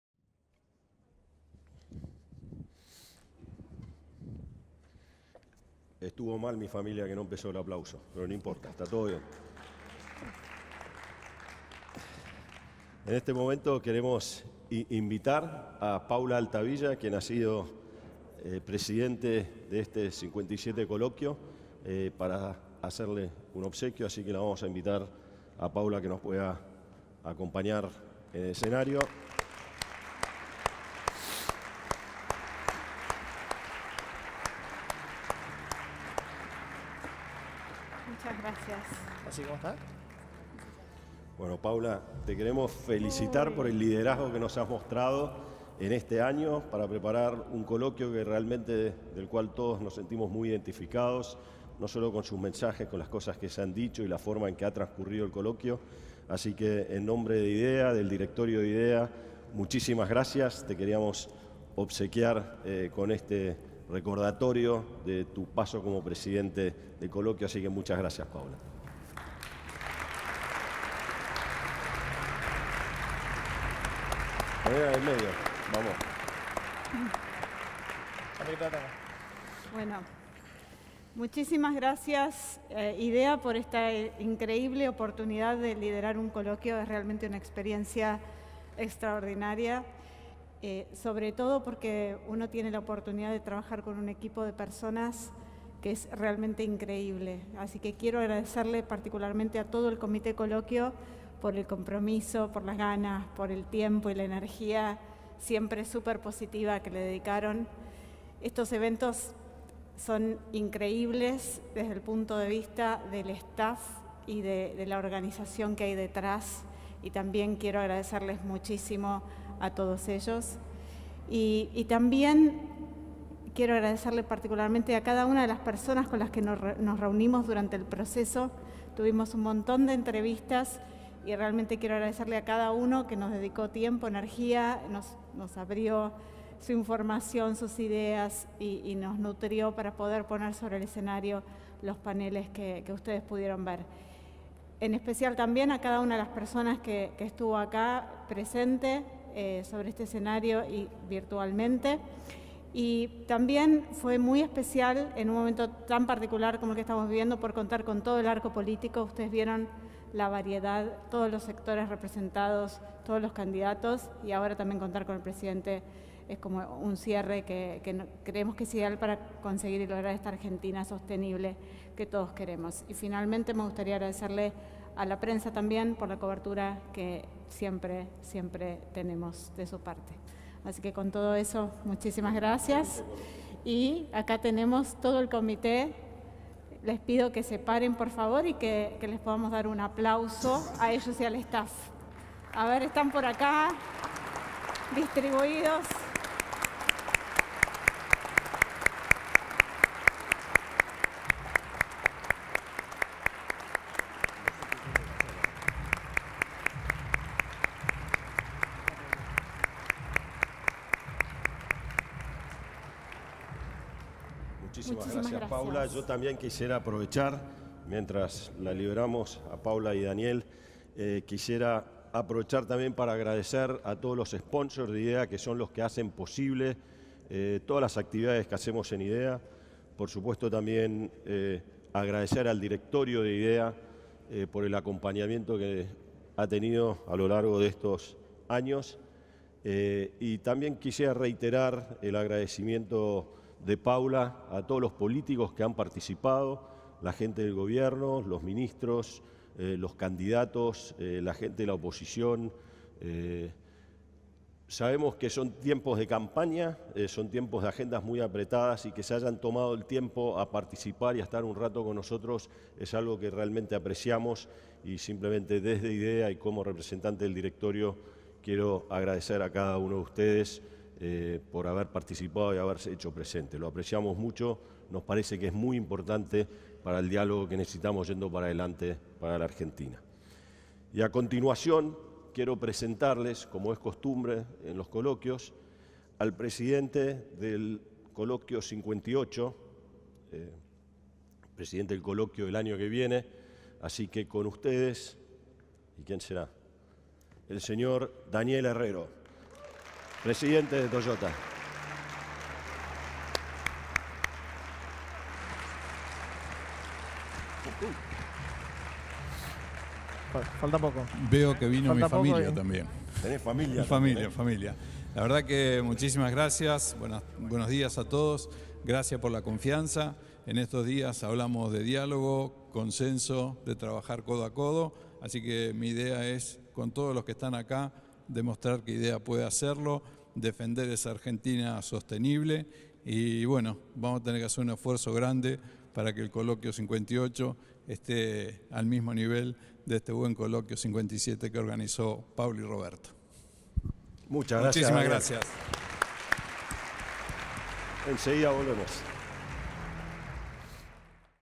57° ColoquioIDEA - Logremos una Argentina sostenible
Expone: Paula Altavilla, Daniel González y Roberto Murchison

Un espacio de reflexión sobre los acuerdos que necesitamos para lograr una Argentina sostenible.

Las empresas tenemos un rol fundamental en la generación de valor y de trabajo de calidad, que necesitamos potenciar. Convocamos a todos los actores sociales a sumarse en un diálogo plural acerca de los temas estructurales que necesitamos abordar para lograr el desarrollo sostenible de la Argentina en el marco de las grandes tendencias globales

Te esperamos el 13, 14 y 15 de octubre para seguirlo en vivo por acá.